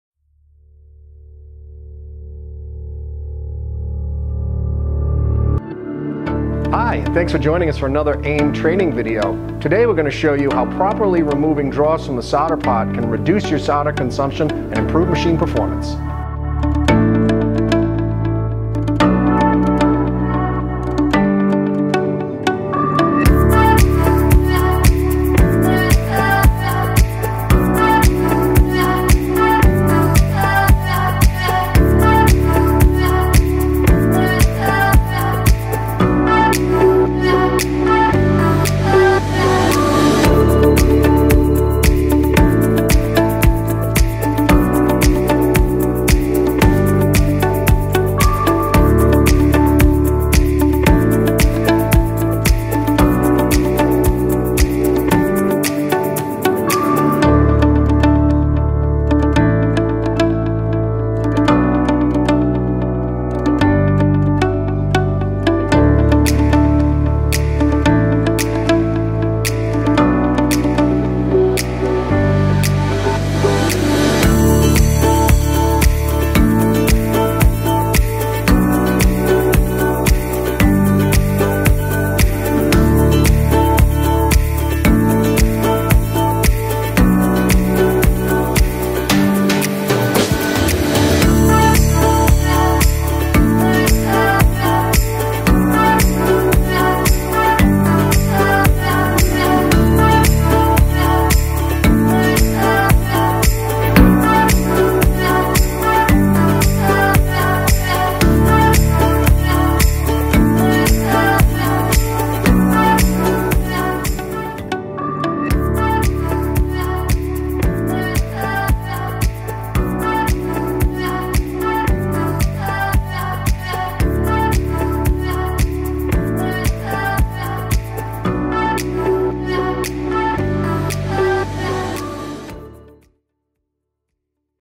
How to Remove Dross from a Solder Pot - AIM Tech Tutorials
Removing dross from the solder pot can reduce your solder consumption and improve machine performance.